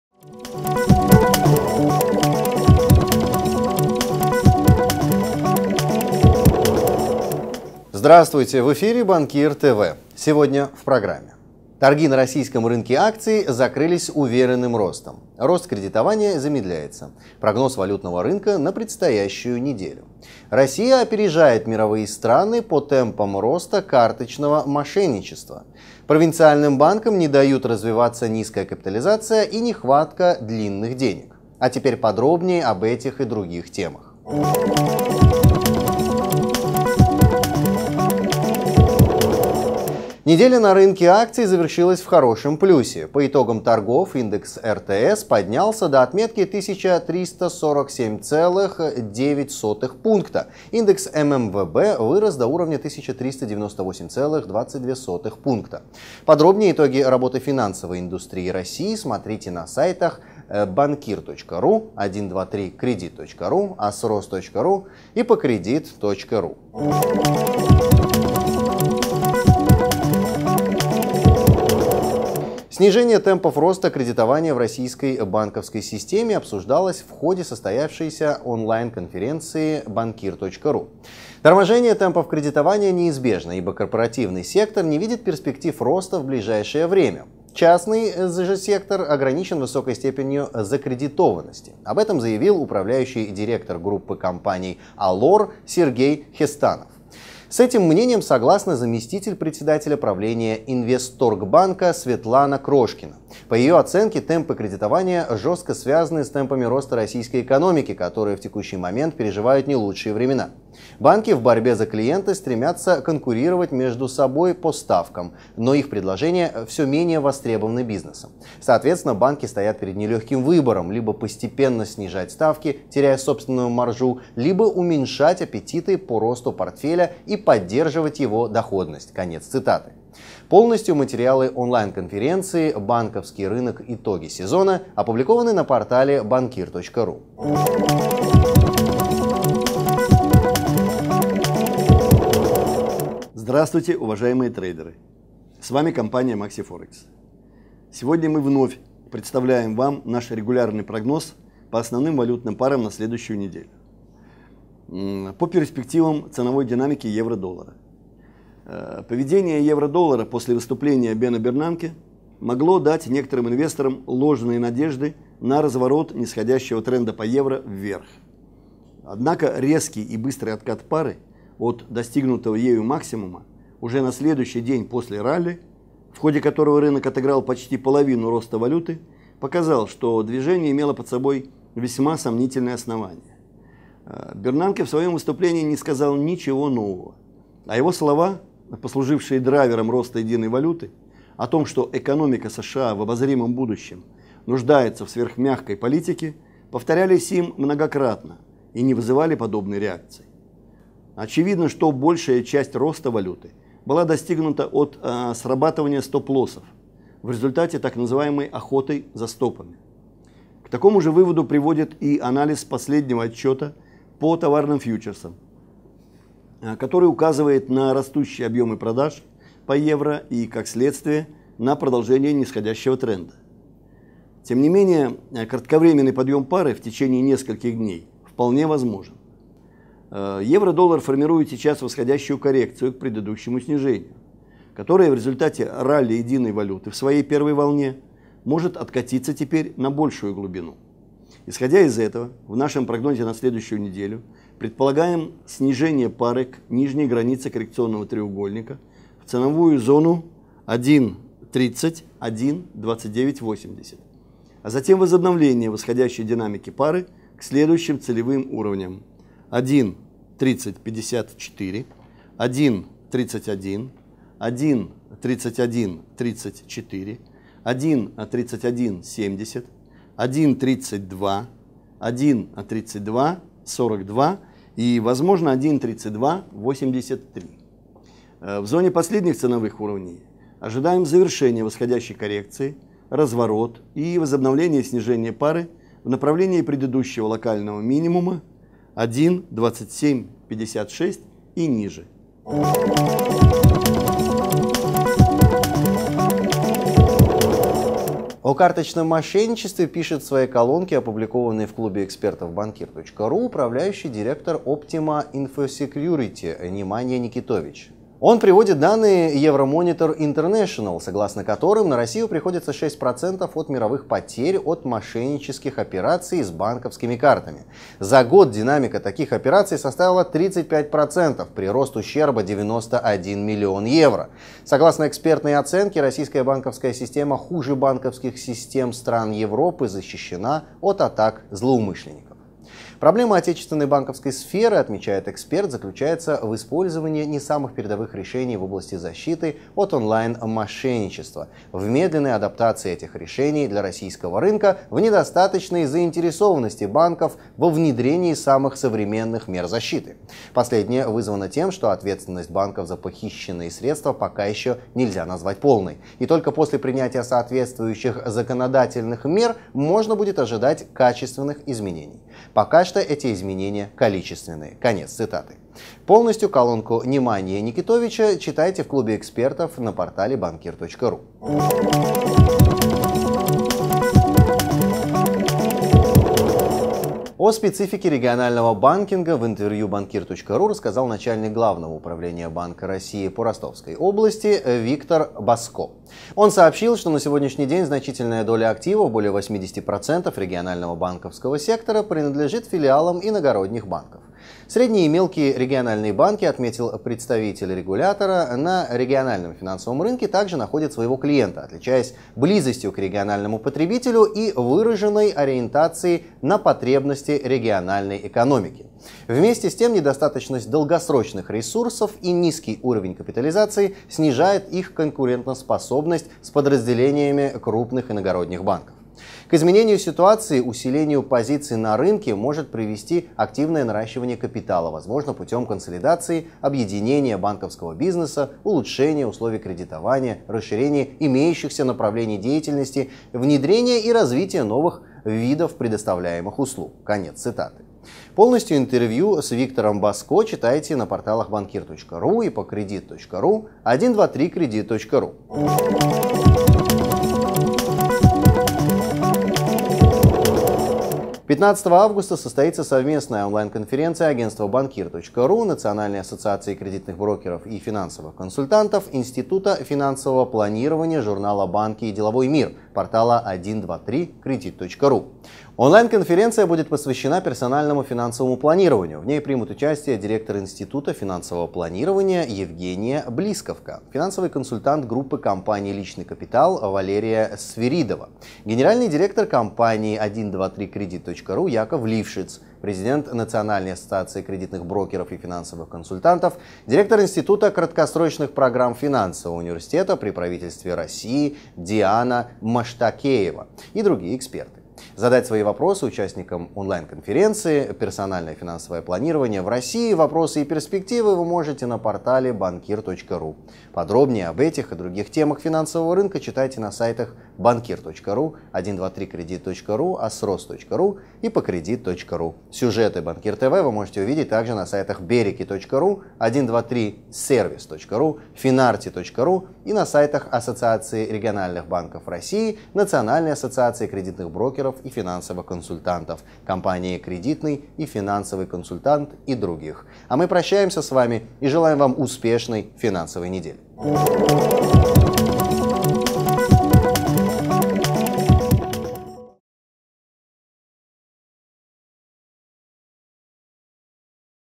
Weekly-15.07.2013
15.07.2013. Bankir-TV-MaxiForex. Бонус до 5000$

Видео обзор финансового рынка России и мирового валютного рынка Форекс (FOREX) от и Банковcкие и финансовые новости, прогноз курсов валют - доллар, евро, фунт... Читайте